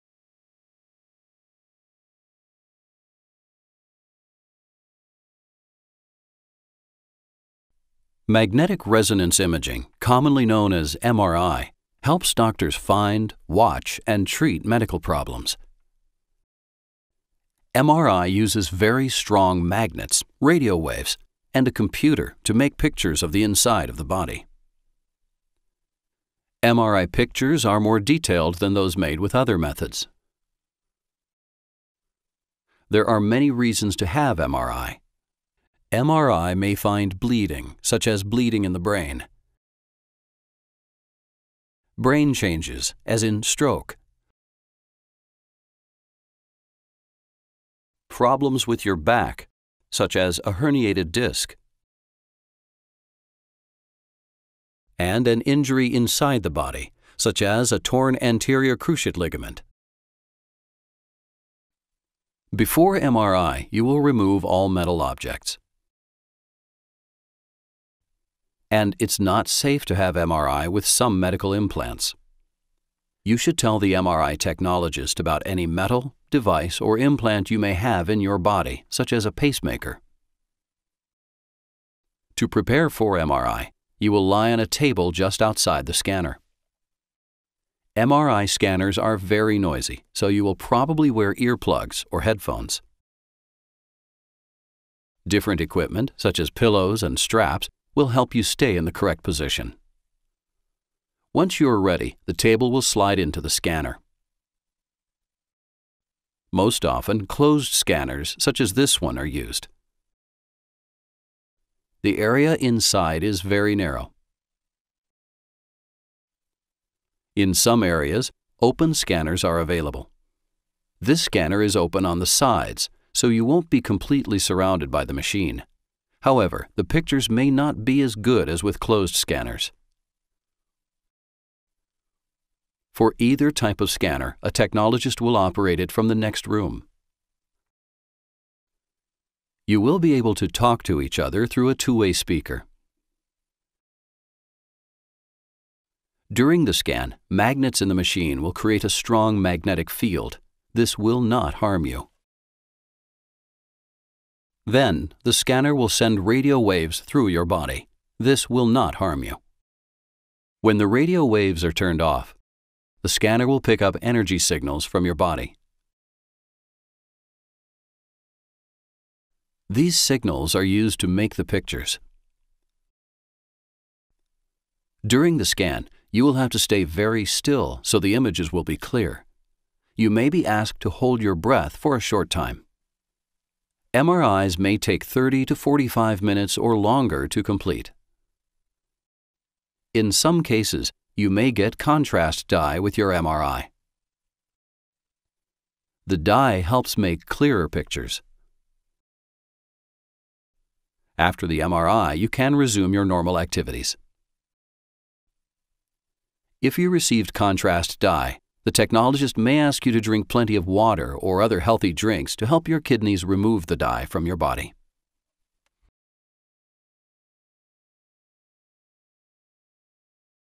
Magnetic Resonance Imaging (MRI)
Magnetic Resonance Imaging, commonly known as MRI, helps doctors find, watch, and treat medical problems. MRI uses very strong magnets, radio waves, and a computer to make pictures of the inside of the body. MRI pictures are more detailed than those made with other methods. There are many reasons to have MRI. MRI may find bleeding, such as bleeding in the brain; brain, changes as in stroke; problems with your back, such as a herniated disk; and an injury inside the body, such as a torn anterior cruciate ligament. Before MRI, you will remove all metal objects. And it's not safe to have MRI with some medical implants. You should tell the MRI technologist about any metal device or implant you may have in your body, such as a pacemaker. To prepare for MRI, you will lie on a table just outside the scanner. MRI scanners are very noisy. So you will probably wear earplugs or headphones. Different equipment, such as pillows and straps, will help you stay in the correct position. Once you're ready, the table will slide into the scanner. Most often, closed scanners, such as this one, are used. The area inside is very narrow. In some areas, open scanners are available. This scanner is open on the sides. So you won't be completely surrounded by the machine. However, the pictures may not be as good as with closed scanners. For either type of scanner, a technologist will operate it from the next room. You will be able to talk to each other through a two-way speaker. During the scan, magnets in the machine will create a strong magnetic field. This will not harm you. Then the scanner will send radio waves through your body. This will not harm you. When the radio waves are turned off, the scanner will pick up energy signals from your body. These signals are used to make the pictures. During the scan, you will have to stay very still, so the images will be clear. You may be asked to hold your breath for a short time. MRIs may take 30 to 45 minutes or longer to complete. In some cases, you may get contrast dye with your MRI. The dye helps make clearer pictures. After the MRI, you can resume your normal activities. If you received contrast dye, the technologist may ask you to drink plenty of water or other healthy drinks to help your kidneys remove the dye from your body.

ANH16182